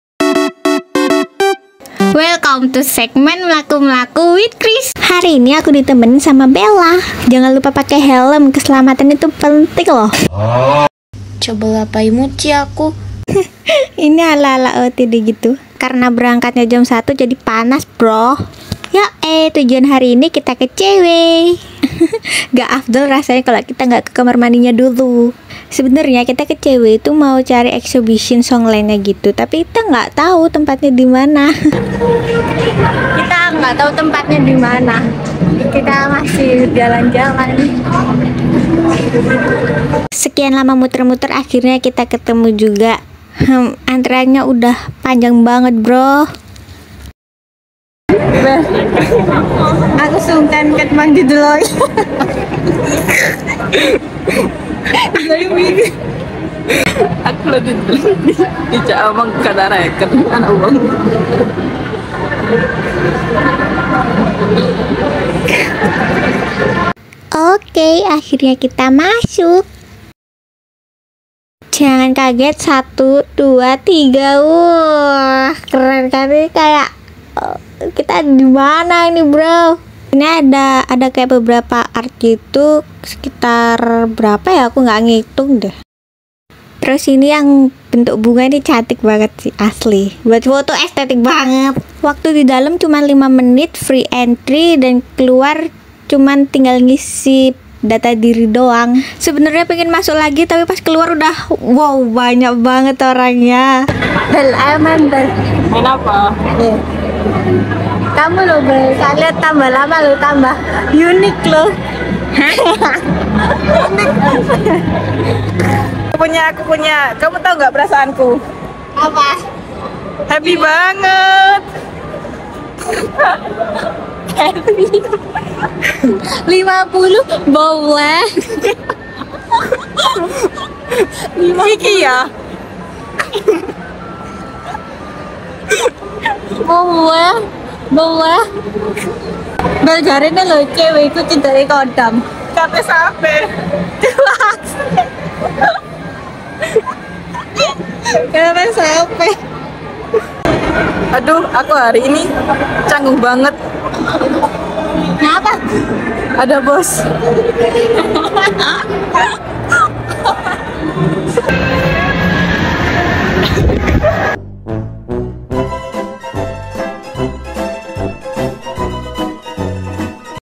Menelusuri isi Mall || Ceilehh akhirnya bikin vlog
Xixixi pertama kali ngevlog malu bangettt, tapi seruuu🤣
Kali ini videonya aku bikin ala"vlog gitu hehe semoga kamu suka yaaaa, langsung nonton videonya okeeee